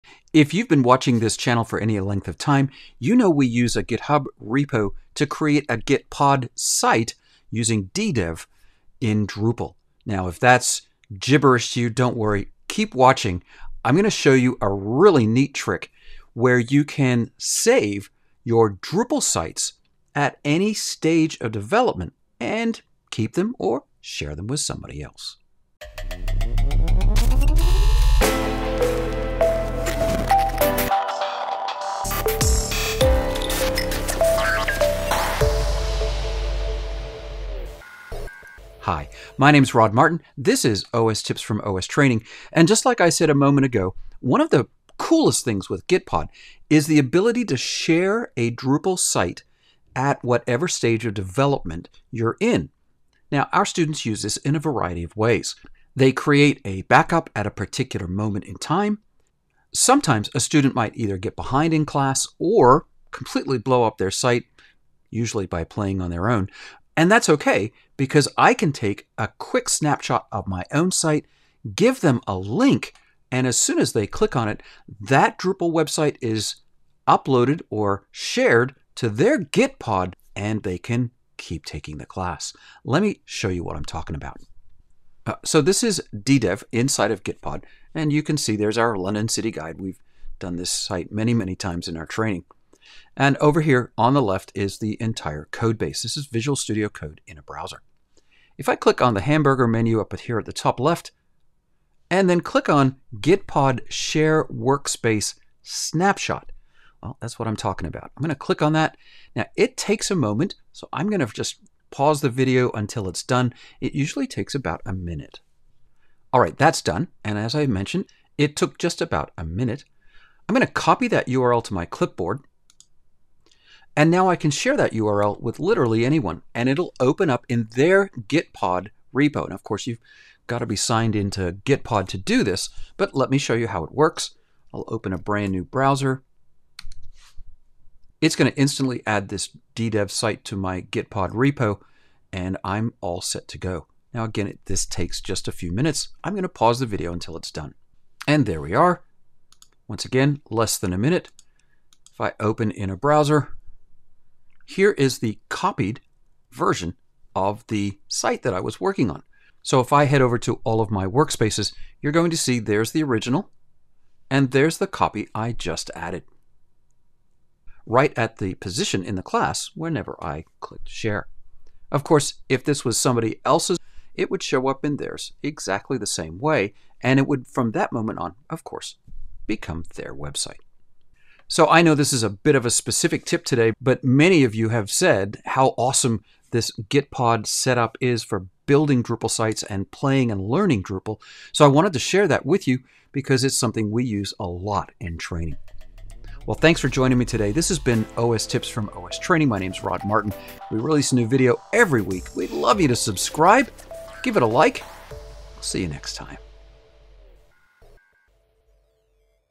How to Share Your Drupal Site at Any Stage of Development in Gitpod
Our students use a Github repository to create as many Drupal websites as they need to take our classes.  This repository creates Drupal sites hosted at GitPod.io and it's really an amazing tool!

Many times, students want to save a copy of the site at a particular stage in the training, or simply share their site with someone else.  As well, if someone gets behind, I can share my site with them and they can continue the class.  This is a life saver!